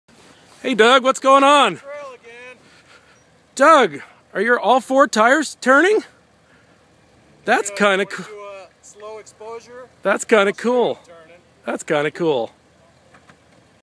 Salem Jeepers Snow Run Jan 2012
Very short video. Wheels moving and going nowhere. Jon gets video credit!